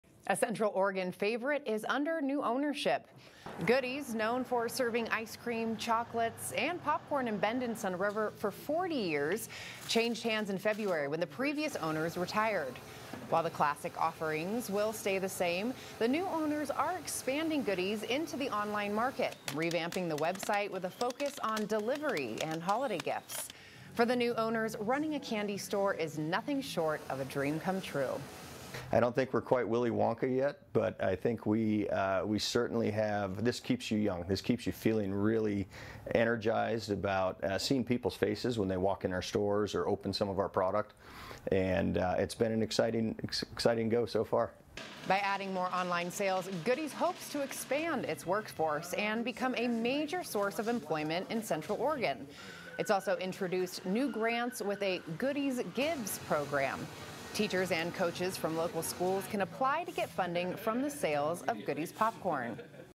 Goody's new owners expand into online market
The new owners of Goody's, the Bend and Sunriver shops featuring ice cream, chocolates and popcorn, have been expanding into the online space, focusing on delivery and holiday gifts.